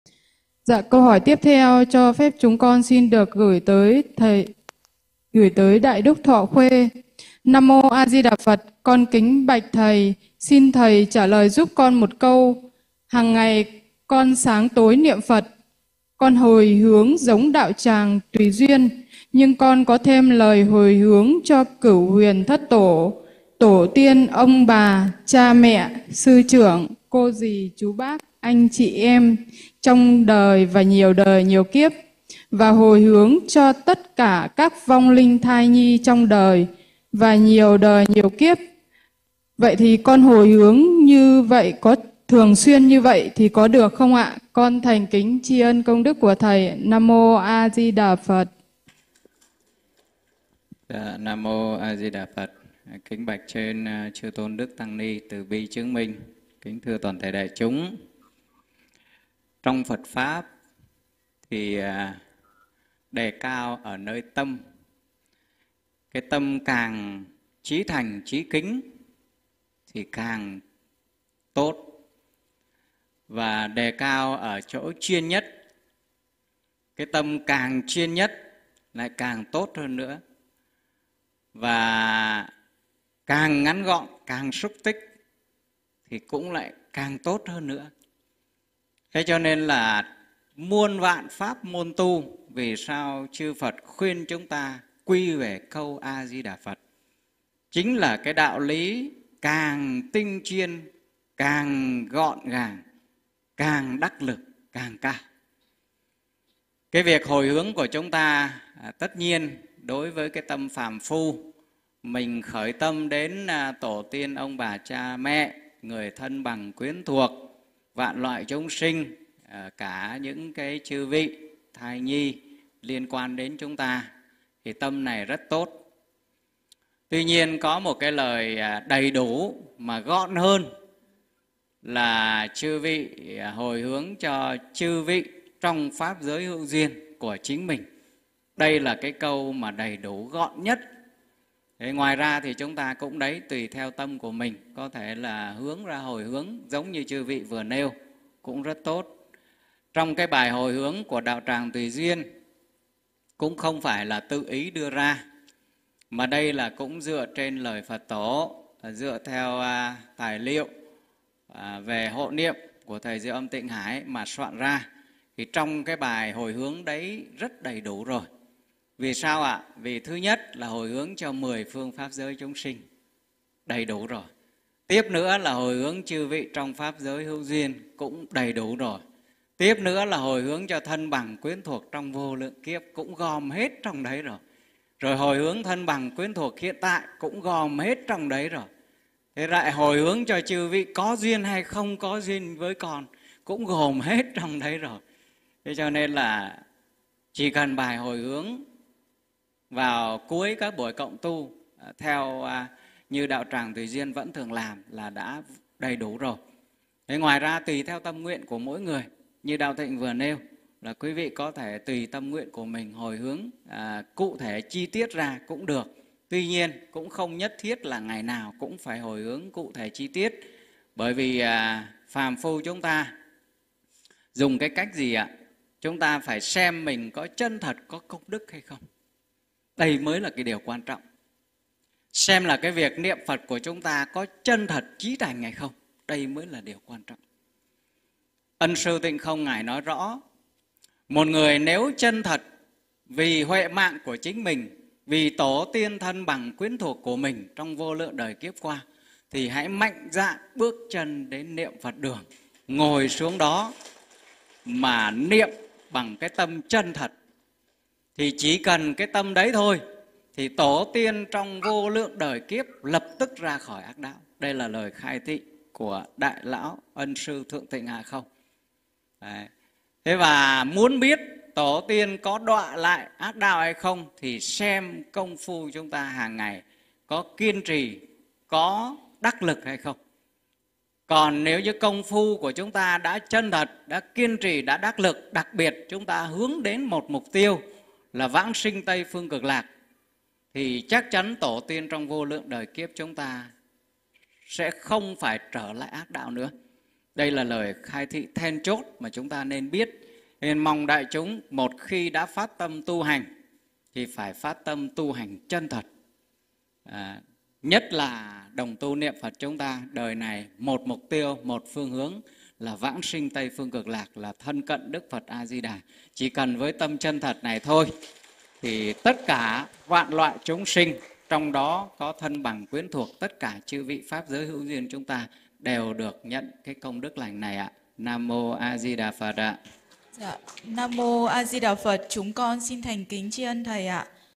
HỒI HƯỚNG CÔNG ĐỨC NHƯ THẾ NÀO CHO ĐÚNG CÁCH? - Đ.Đ THÍCH ĐẠO THỊNH CHIA SẺ
Trích đoạn Đại đức Thích Đạo Thịnh chia sẻ, trả lời câu hỏi của đồng tu trong buổi Cộng tu Niệm Phật chiều Chủ Nhật ngày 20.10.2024